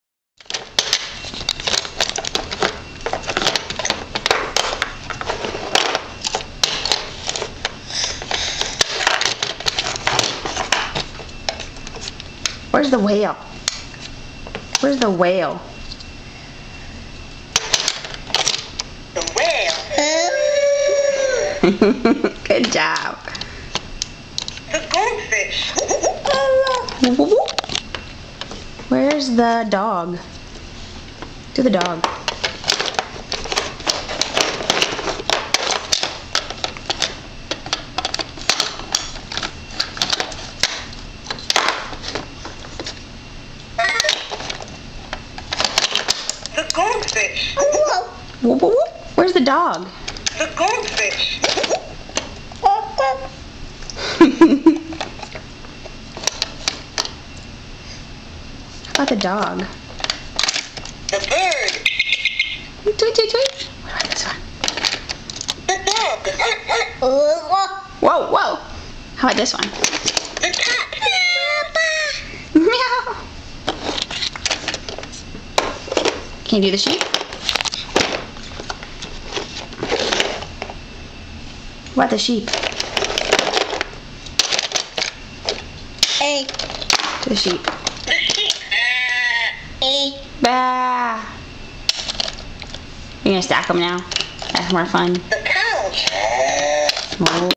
AJ and animal cubes
This is AJ working on his animal sounds.